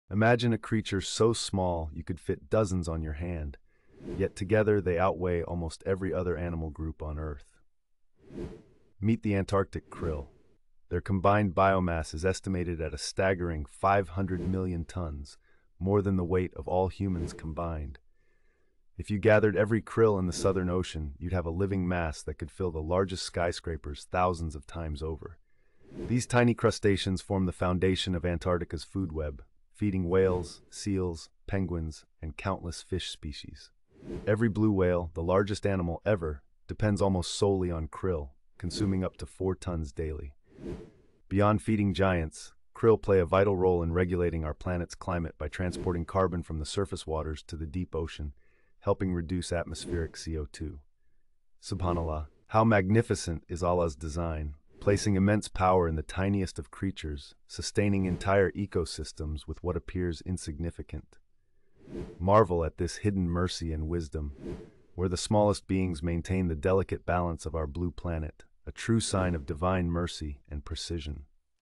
Antarctic Krill: Tiny Creatures, Massive Impact
Discover the mind-blowing power of Antarctic krill! Their numbers are astonishing and their role in the ocean is miraculous.